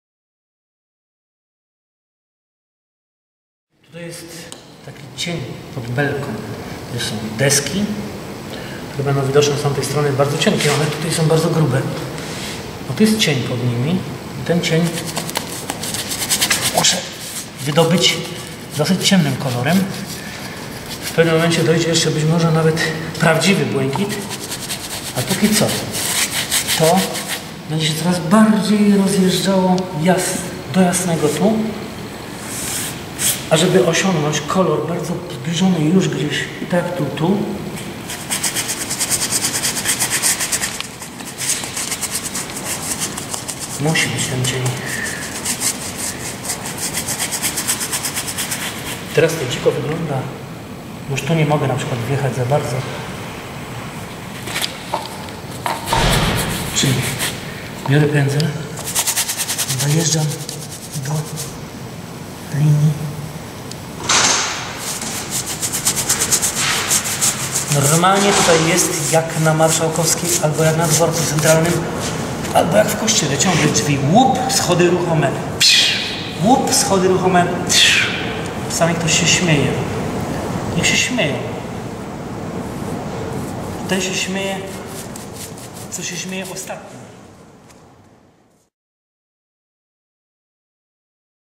THE UNDERGLASS street-art LIVE! - Ten się śmieje kto się śmieje ostatni
Ryszard "Ryho" Paprocki o pracy nad street-artowym projektem "The UnderGlass - street-art LIVE!", w ramach którego w krośnieńskim Centrum Dziedzictwa Szkła powstaje malowidło 3d.